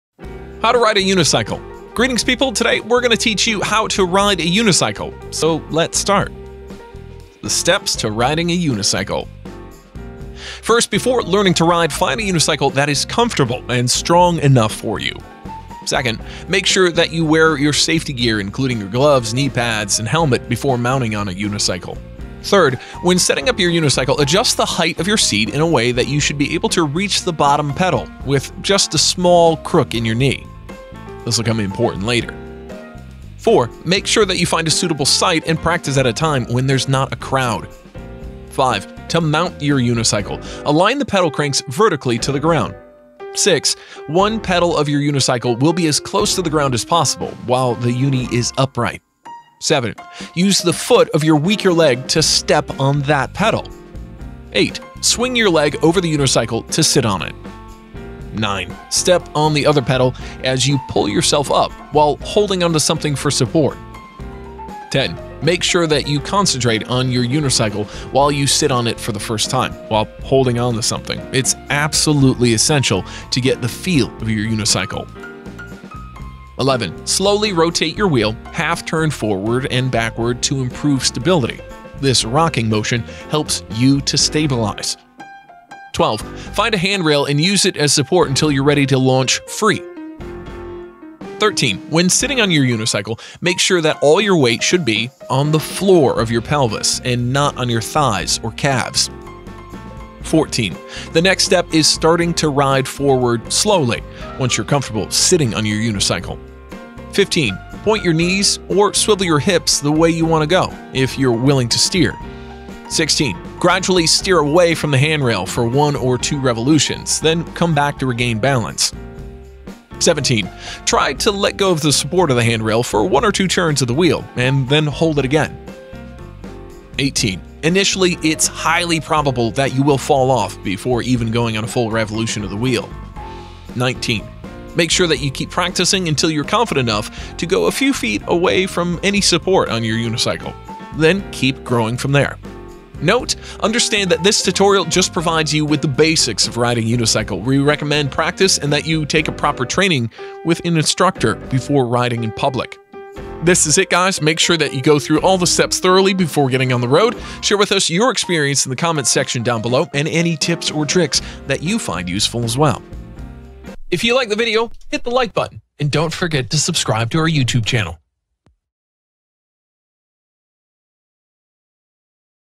Learn How to Ride a Unicycle for Beginners in 3 Minutes?
Today we are going to teach you how to ride a UNICYCLE.
Before learning to ride, find a unicycle that is comfortable and strong enough for you.
Make sure that you wear your safety gear including your gloves, knee pads, and helmet before mounting on your unicycle.
When setting up your unicycle, adjust the height of your seat in a way that you should be able to reach the bottom pedal, with just a small crook in your knee.
Make sure that you find a suitable site and practice at a time when there is not enough crowd.

NOTE: Understand that this tutorial just provides you with the basics of riding a unicycle.
We recommend that you take proper training with an instructor before riding it publicly.